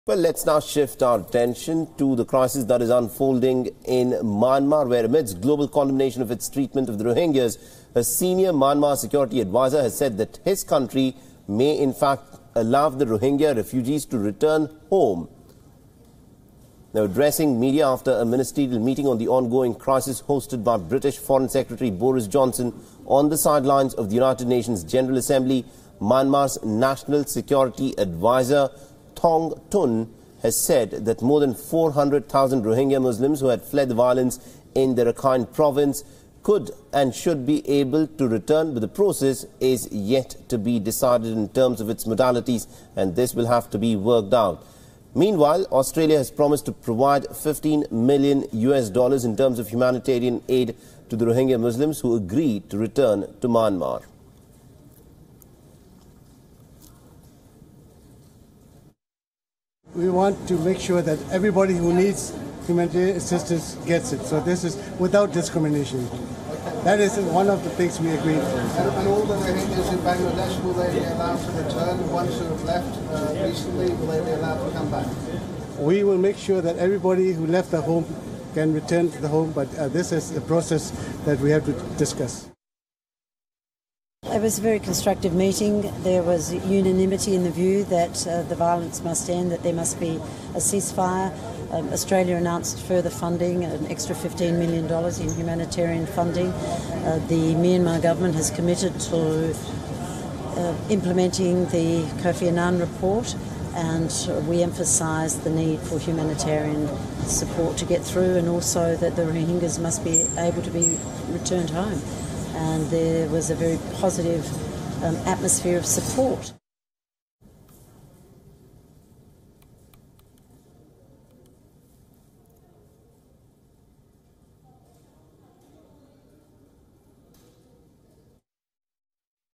Myanmar agrees for Rohingya return?
Myanmar National Security Advisor says, "Rohingya refugees may be able to return." Myanmar officials says process for return of Rohingya refugees yet to be declared.